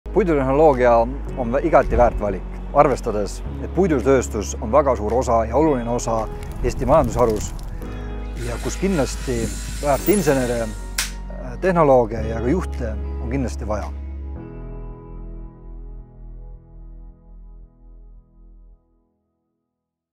Puidu-, plasti- ja tekstiilitehnoloogia magistrikava: Puidutehnoloogia
Puidutehnoloogia suunal õpetatavad puidutööstuse, mööblitööstuse, pehme mööbli ja puidust tehasemajade tehnoloogia õppeained on Eestis unikaalsed, sest nende õpetamisse on kaasatud praktiseerivad tootmisspetsialistid. Mööbli- ja pehmemööbli projektide raames tegeled praktiliste tootearendus- või tootmisprobleemide lahendamisega, mis nõuab mitmekülgseid teadmisi ja valmistab üliõpilasi tööeluks hästi ette.